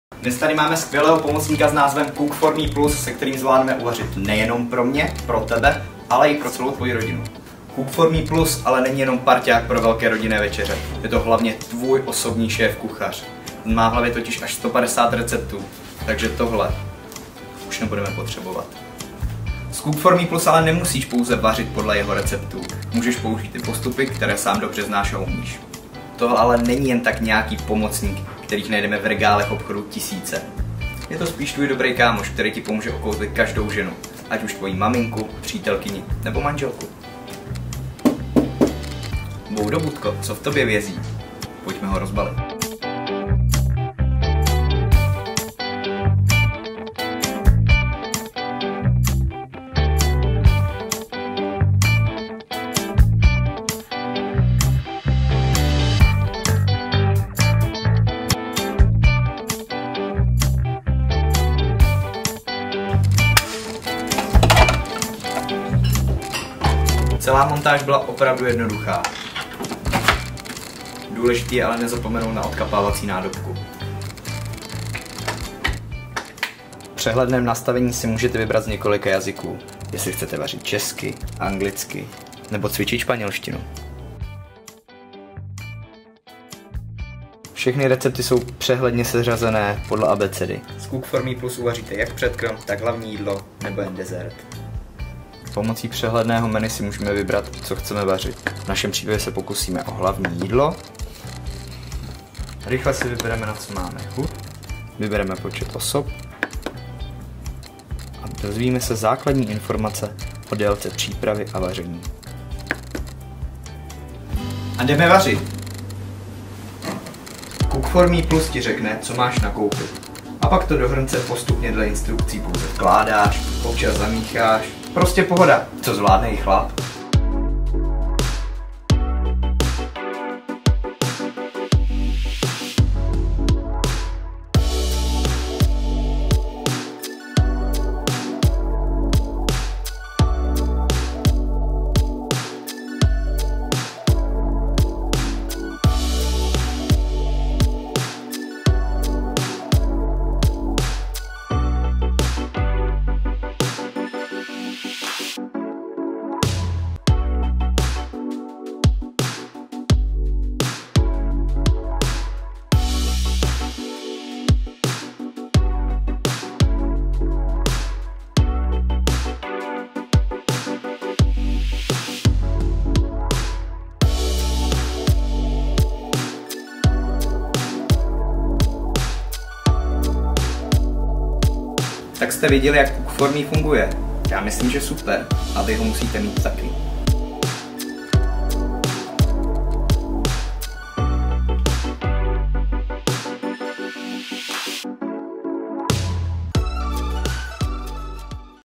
Tefal Cook4me+ - Bemutató
A Cook4me+ többfunkciós edény intuitív és intelligens kezelőpanellel ellátott, ügyes segítőtárs. Válasszon a 6 főzési mód közül és hagyja, hogy a kukta végigvezesse a 150 előre beprogramozott recept valamelyikén.